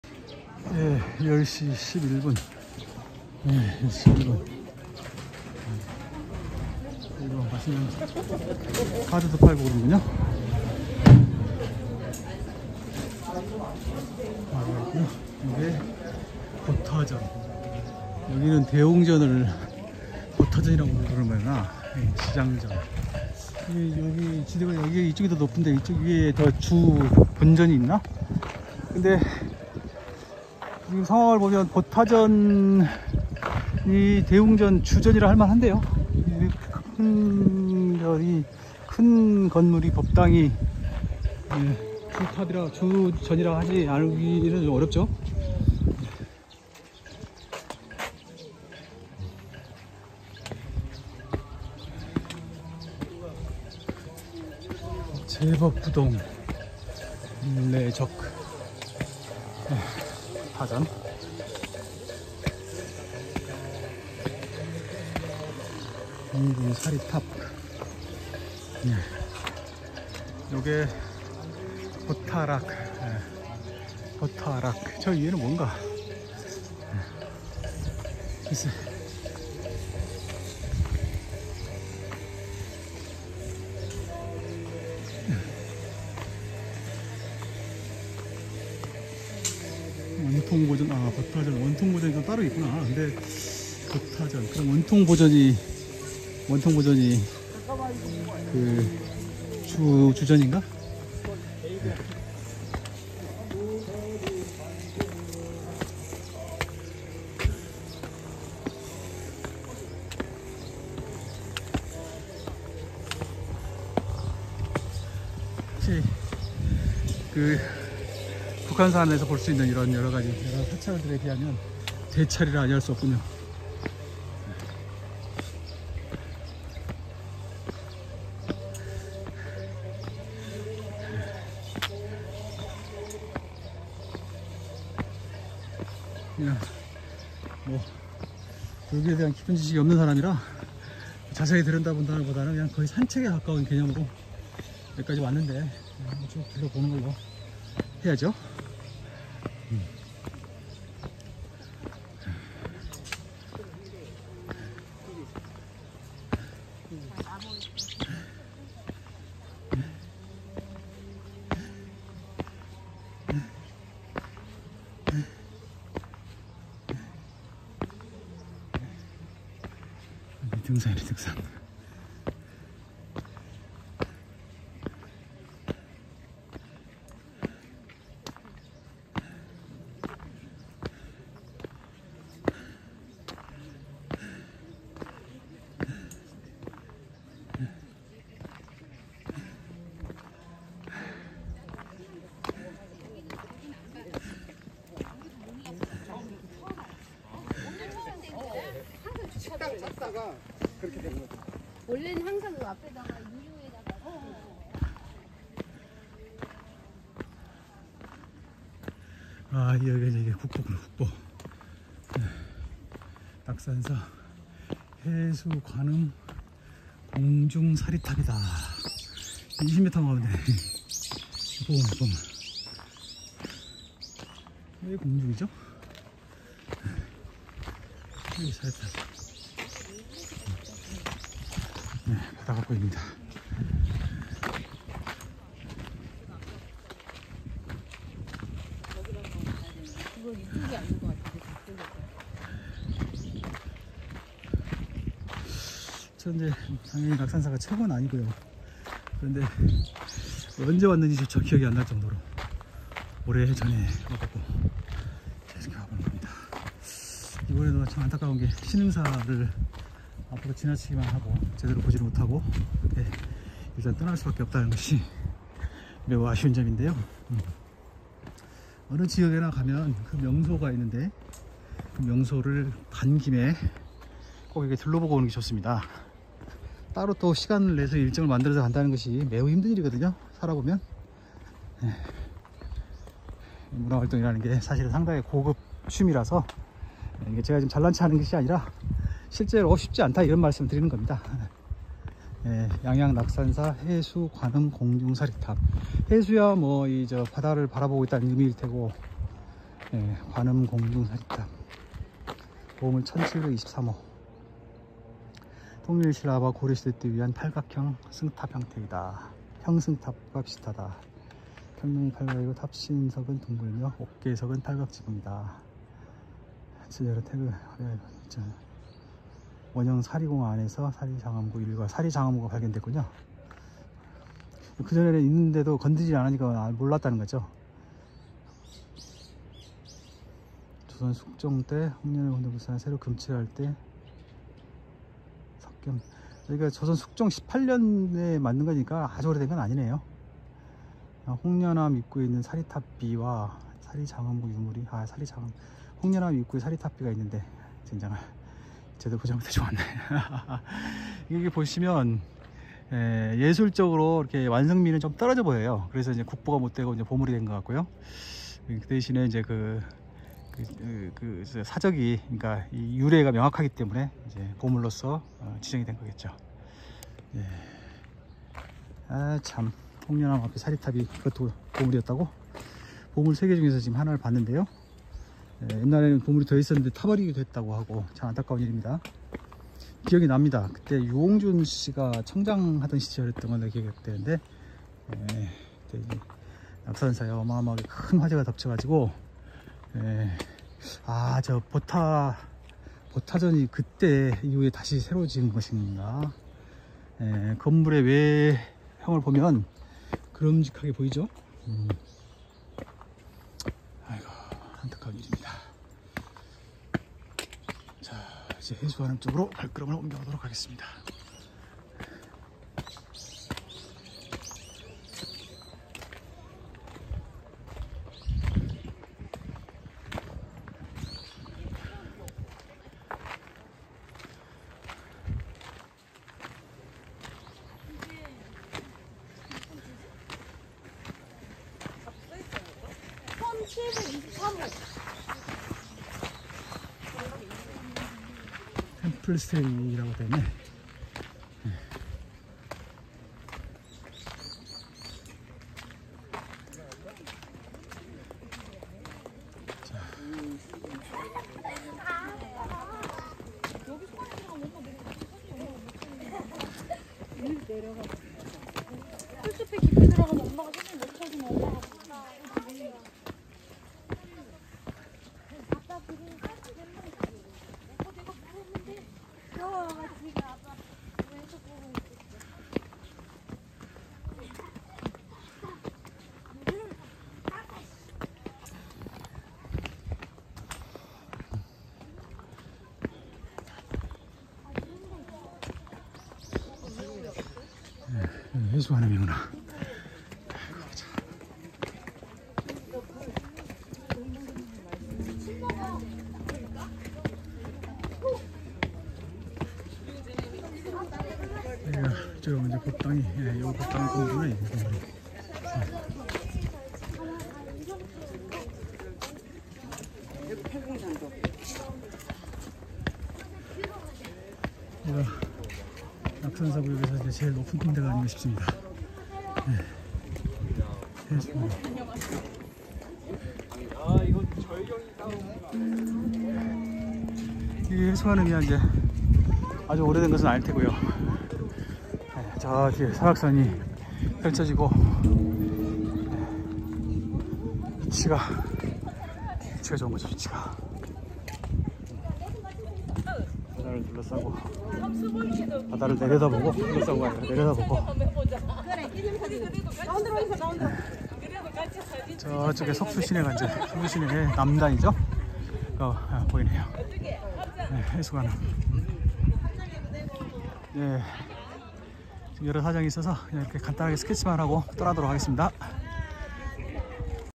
(55)(설악산) 낙산사 보타전과 해수관음을 살펴보다.
안타깝게도 시간이 많지 않아 번개불에 콩구어 먹듯이 둘러봅니다. 거찰의 면모가 엿보입니다. 보물도 여러 점이었습니다.